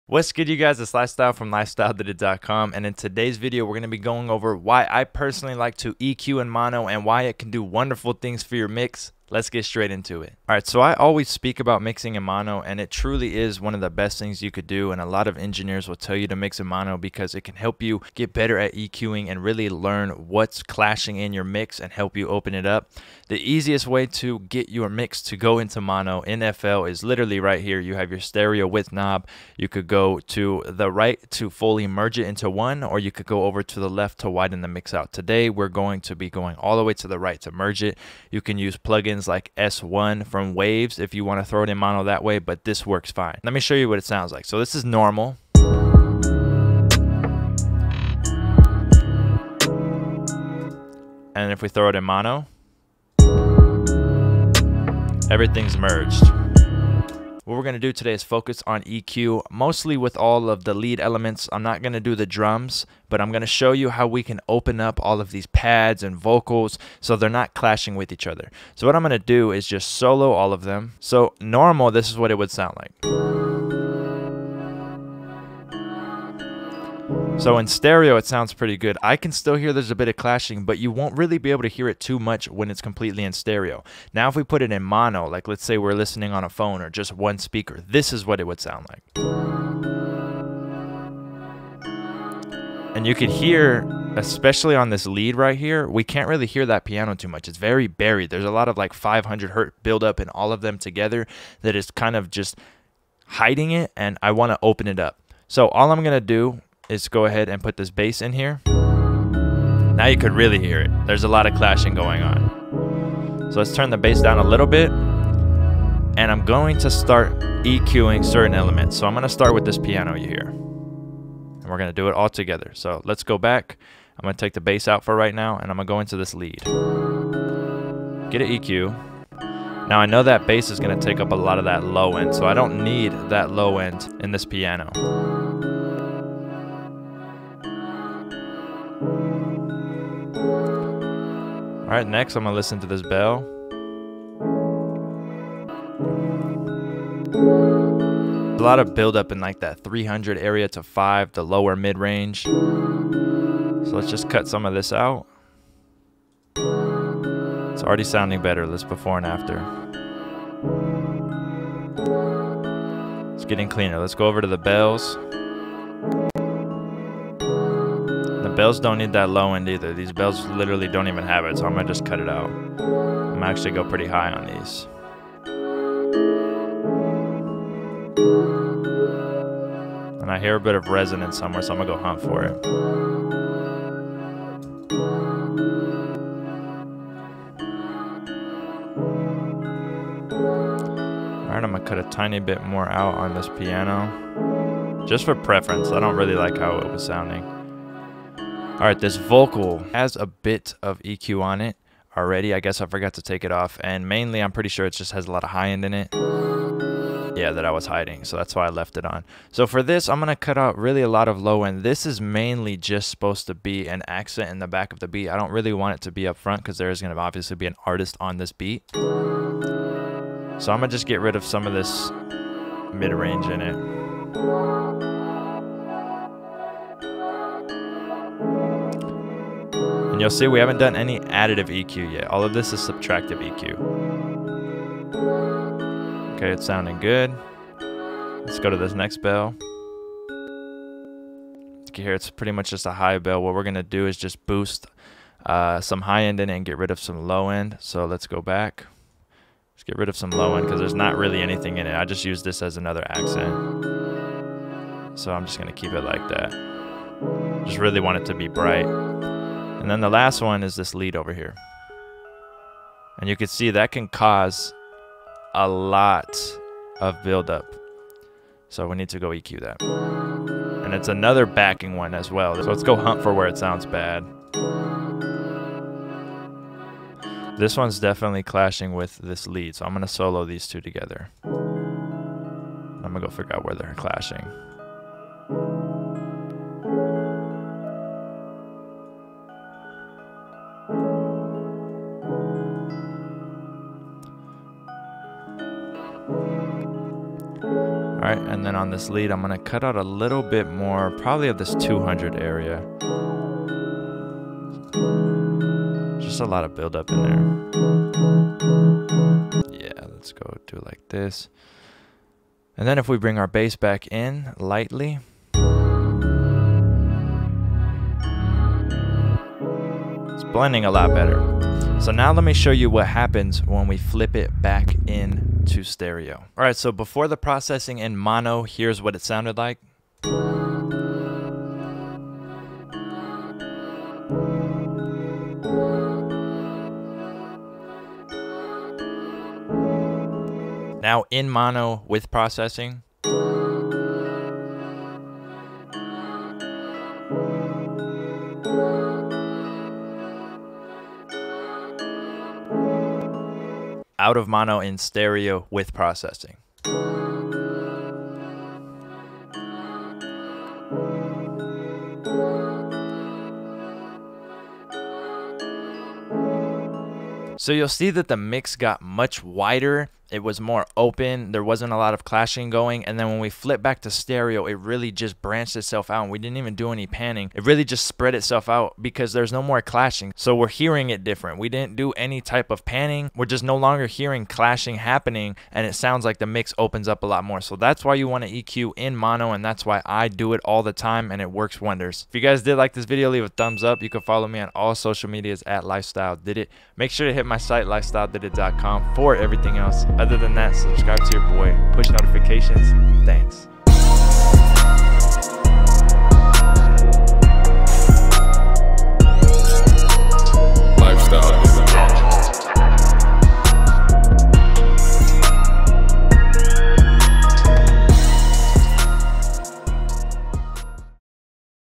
WHY YOU SHOULD EQ YOUR MIX IN MONO
In this video, I show the benefits of EQ'ing your mix in mono!

Upload your beats/songs to itunes, Spotify, etc. with distrokid and save when you use my link: